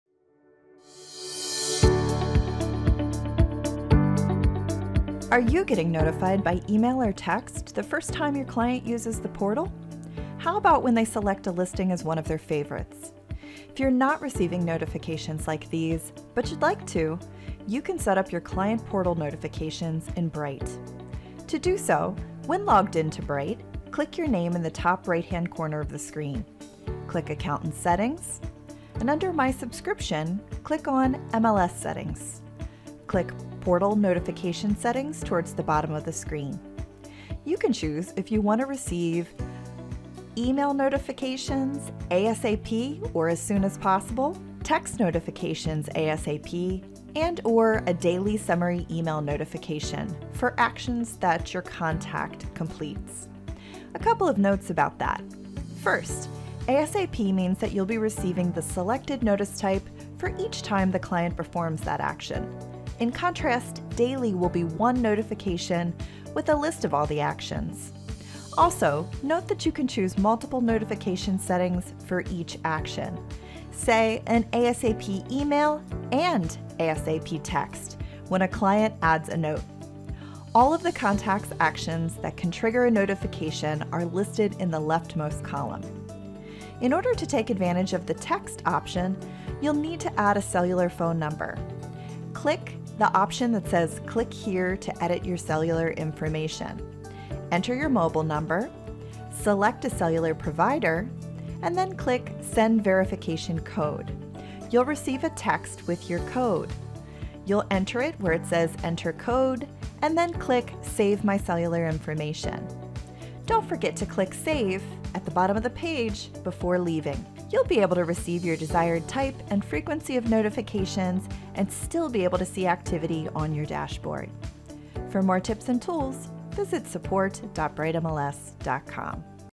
Setting Portal Activity Notifications
Are you getting notified by email or text the first time your client uses the portal? How about when they select a listing as one of their favorites? If you are not receiving notifications like these, but you’d like to, you can set up your Client Portal notifications in Bright.
To do so, when logged into Bright:
Click your name in the top right-hand corner of the screen
Click Account & Settings.
Under My Subscription.
Click MLS Settings.
Click Portal Notification Settings toward the bottom of the screen.
You can choose if you want to receive ASAP (as soon as possible) via Email, ASAP via Text and/or Daily Summary email notifications for actions by your contact A couple of notes about that: first asap means that you’ll be receiving the selected notice type for each time the client performs that action, in contrast Daily will be one notification with a list of all the actions. Also, note that you can choose multiple notification settings for each action, say an ASAP Email and text when a client adds a note). All of the contact’s actions that can trigger a notification are listed in the left most column.
In order to take advantage of the text options you’ll need to add a cellular phone number:
Click Click Here to edit your cellular information.
Enter Mobile Phone Number.
Select a Cellular Provider.
Click Send Verification Code. You will receive a verification code by text.
Enter the Verification Code where it says Enter Code.
Click Save my Cellular information.
Don’t forget to Click Save before leaving the page.
You’ll be set to receive your desired type and frequency of notifications and still be able to see activity on your dashboard.
For more tips and tools, visit support.brightmls.com.

Have a question, or need some help with Bright MLS?
Call our Customer Support Center
Toll Free: 1-844-55-BRIGHT
TRN-807